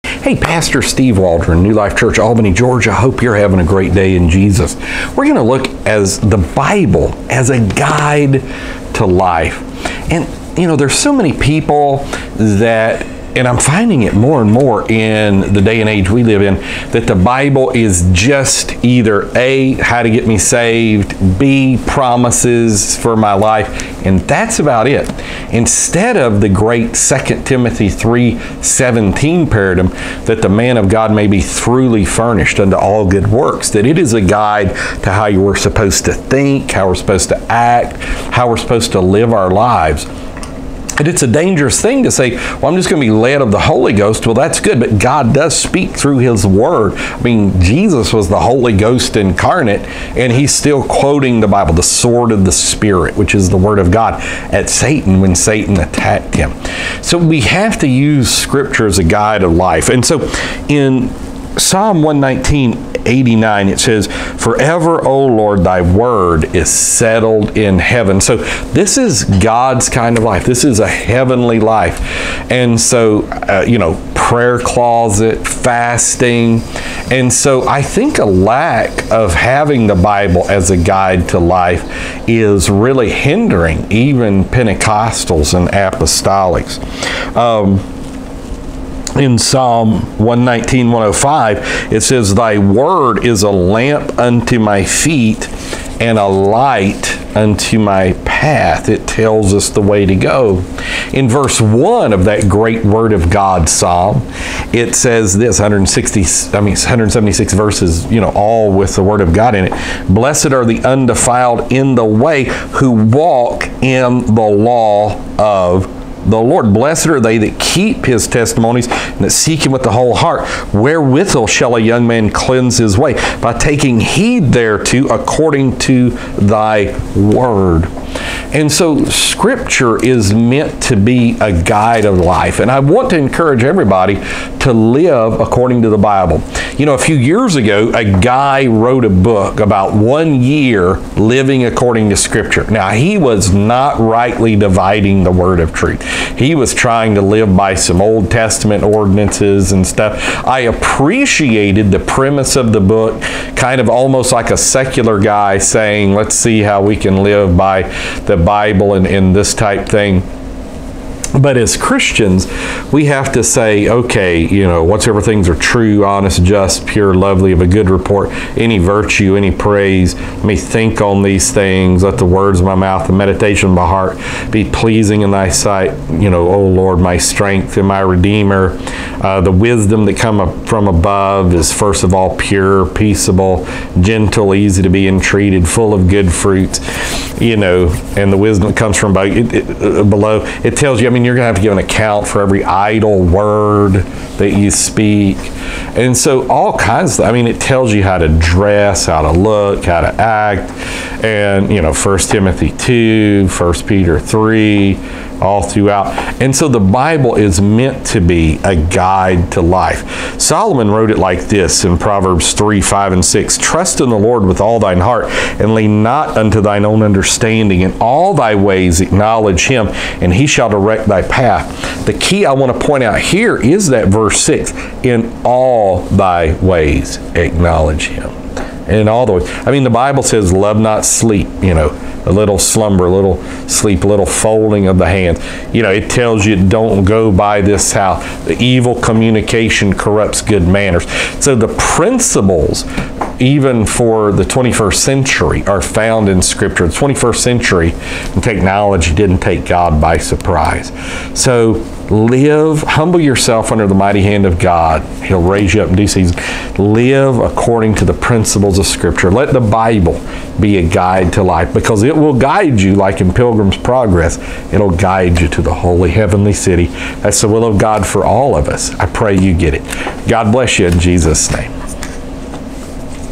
The Bible As A Guide To Life!
To contribute to New Life, please click the link below and press donate. God bless!  The Bible contains all things that pertain to life and Godliness.  The Christian has a road map!